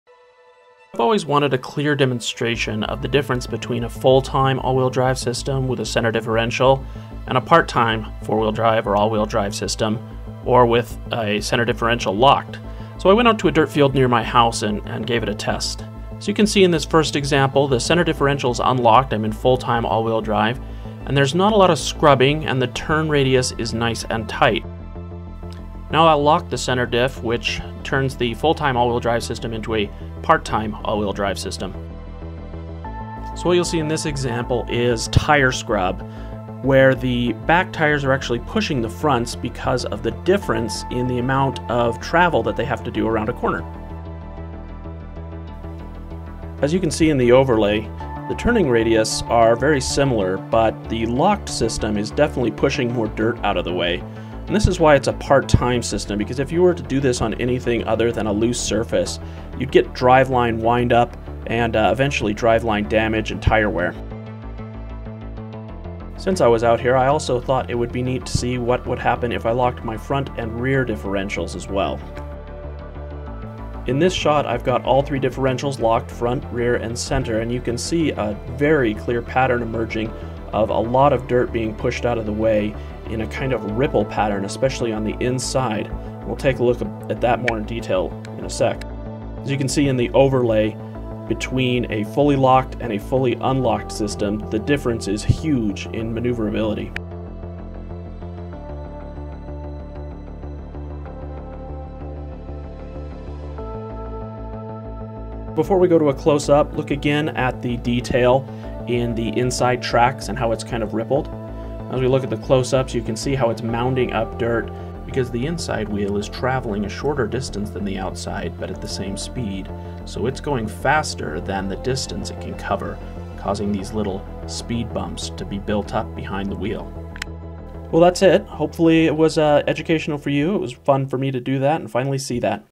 What AWD scrub looks like
I've always wanted a clear visual for tire scrub in AWD full-time and AWD part-time or locked, so I went out and captured it.